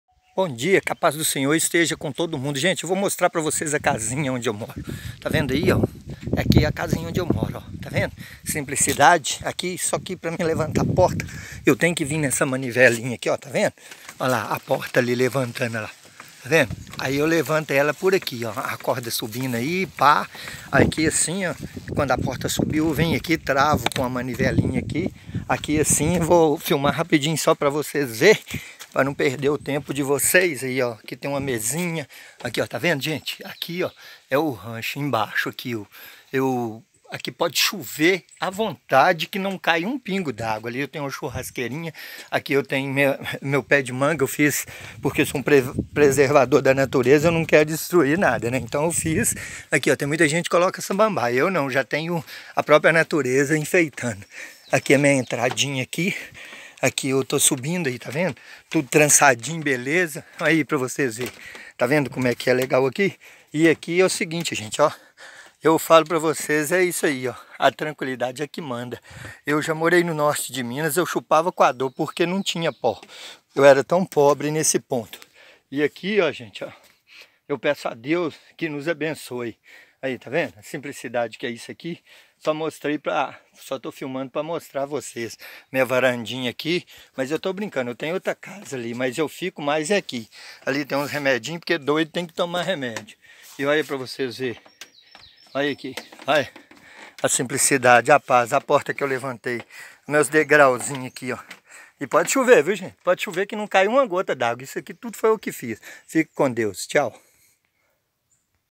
Pompeano vive em casa na árvore no Aterrado.
Vídeo de Experidiao Porto